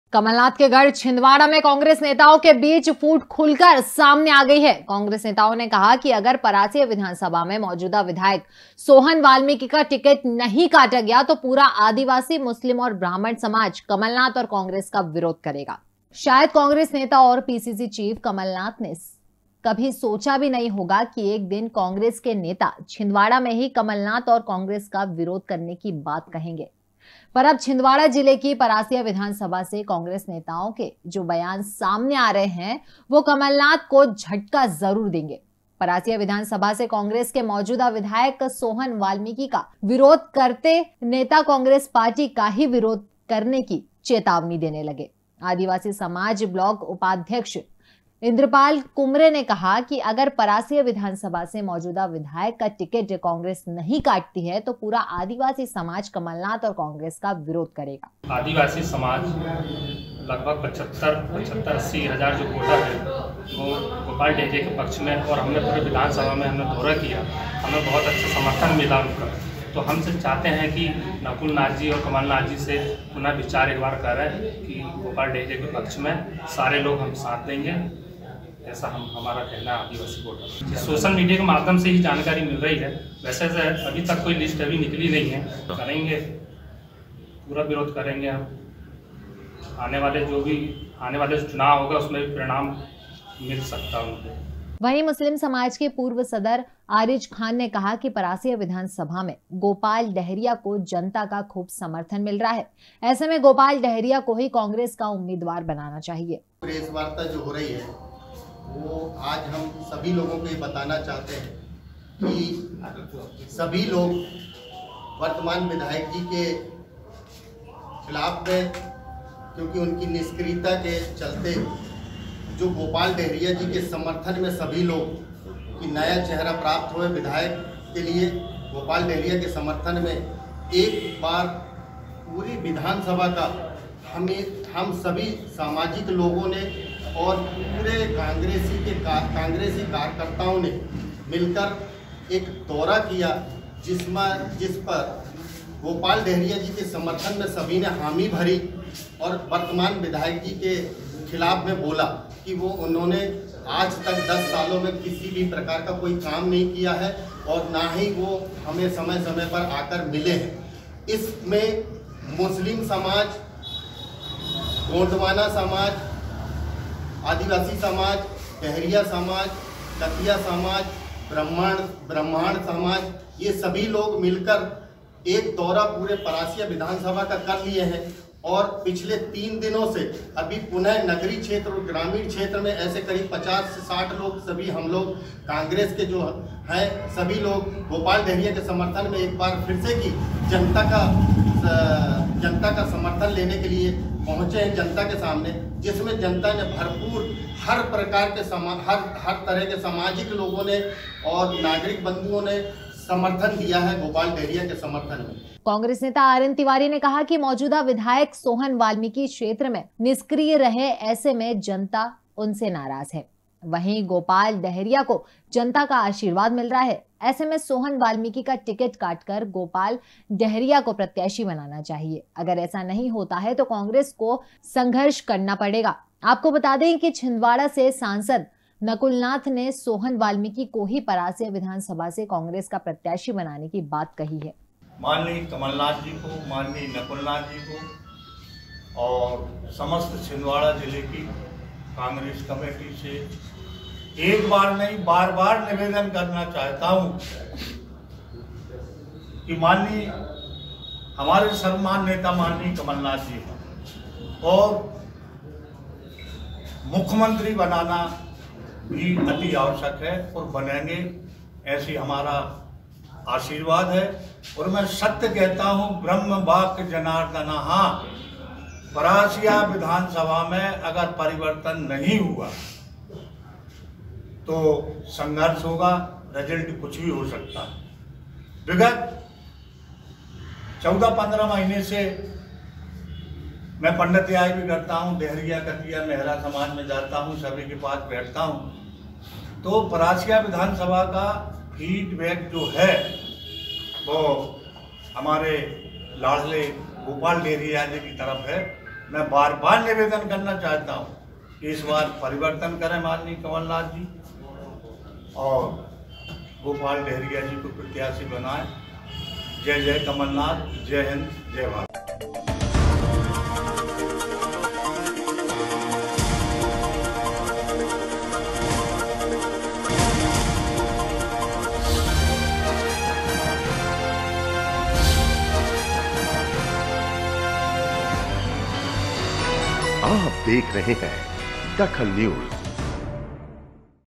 DAKHAL NEWS परसिया में कांग्रेस नेताओं की फूट सामने आयी
परसिया में कांग्रेस नेताओं की फूट सामने आयी
विधायक सोहन वाल्मीकि की टिकट काटने की मांग